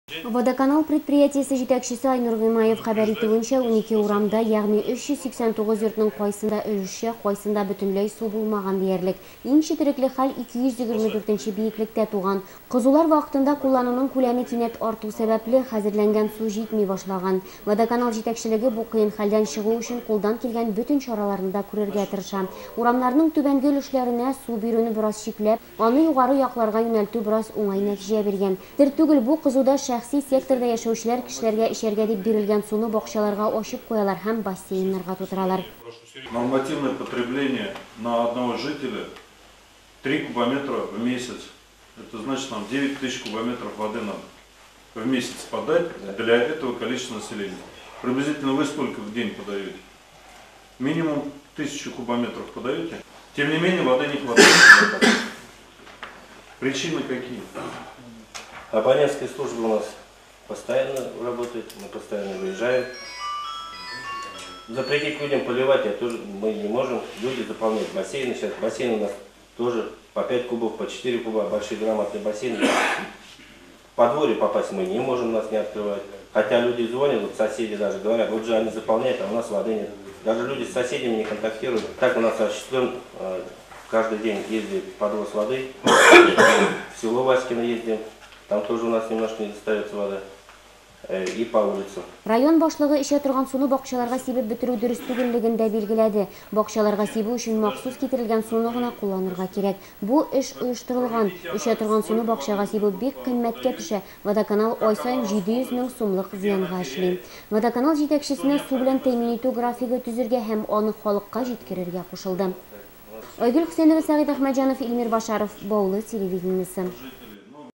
Аппарат киңәшмәсе
Дүшәмбе көнне район башлыгы Рамил Гатиятуллин үткәргән аппарат киңәшмәсе нигездә кызулар аркасында шәһәрдә туган су җитмәүчелек мәсьәләсен чишү юлларын эзләүгә багышланды.